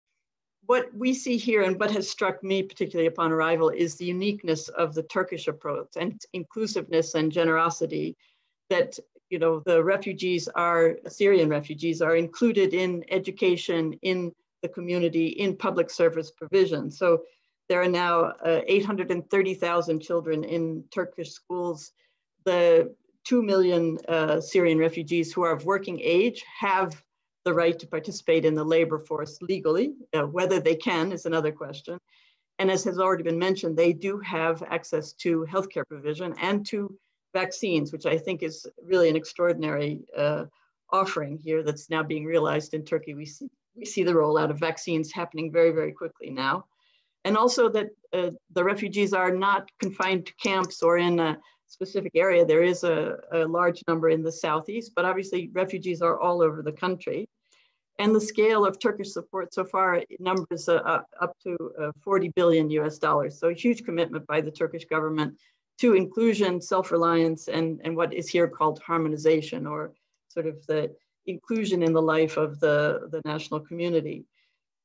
Vinton praises Turkey for refugee policies
Turkey’s UNDP representative Louisa Vinton says Turkey gives refugees access to  education, healthcare, and jobs.